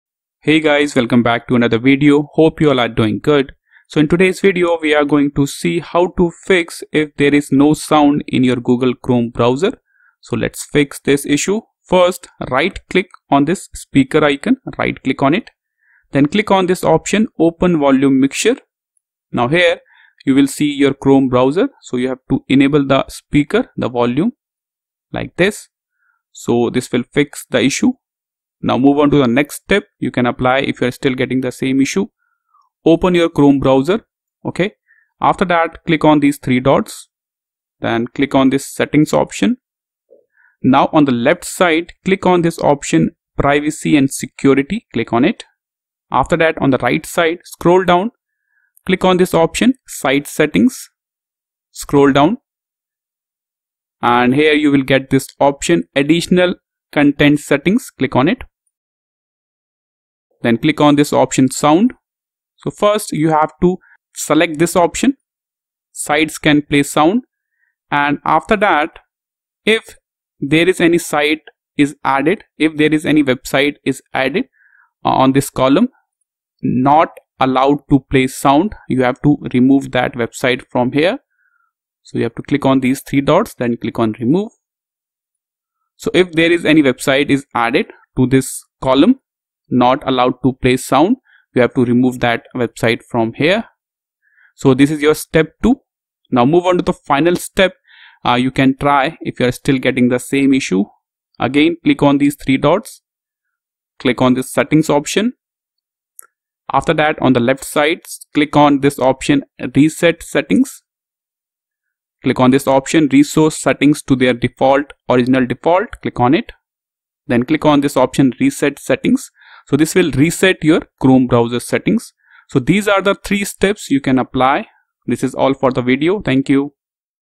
How To Fix No Sound in Google Chrome - (3 Ways to Fix)
How To Fix No Sound In Google Chrome
Chrome Sound Not Working in Windows